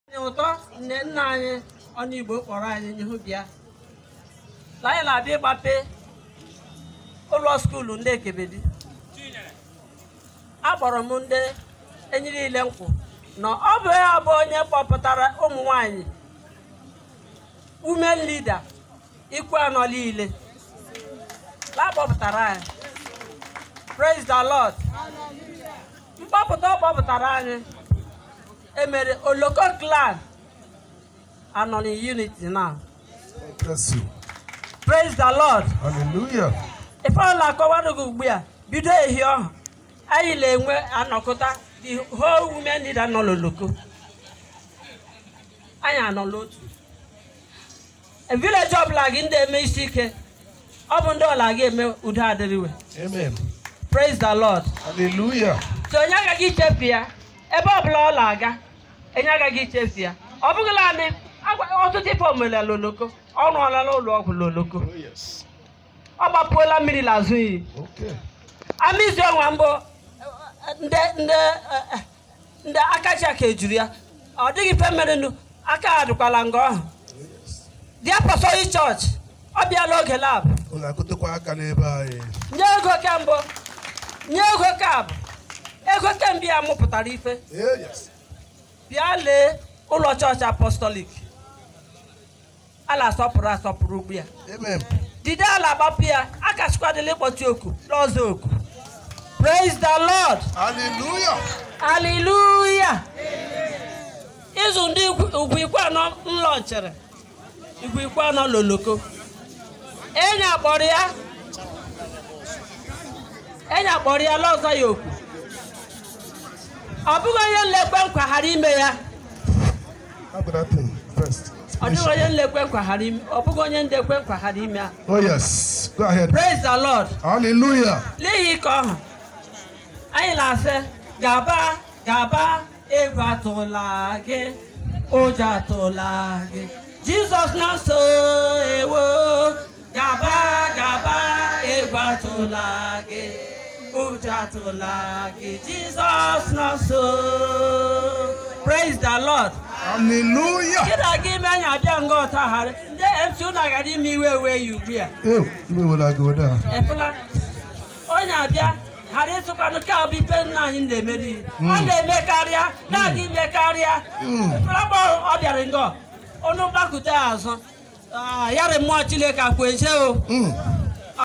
Presentation by Ugwu Ikwuano Women Leaders Association
Commissioning of a block of four classrooms at Ekebedi Oboro, Ikwuano LGA, Abia State.
Rep. Sam Onuigbo
Member representing Ikwuano/Umuahia North/South Federal Constituency, Abia State.